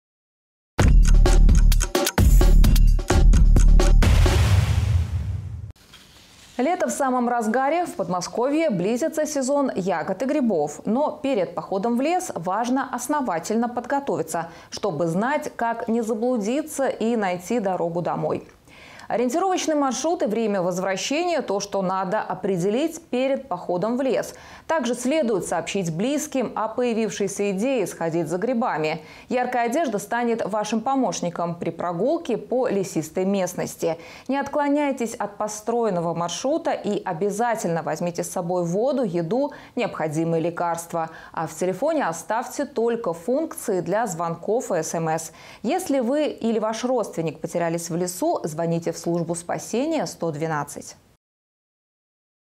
НОВОСТИ ПОДМОСКОВЬЯ
Лето в самом разгаре, в Подмосковье близиться сезон ягод и грибов. Но перед походом в лес, важно основательно подготовиться, чтобы знать как не заблудиться и найти дорогу домой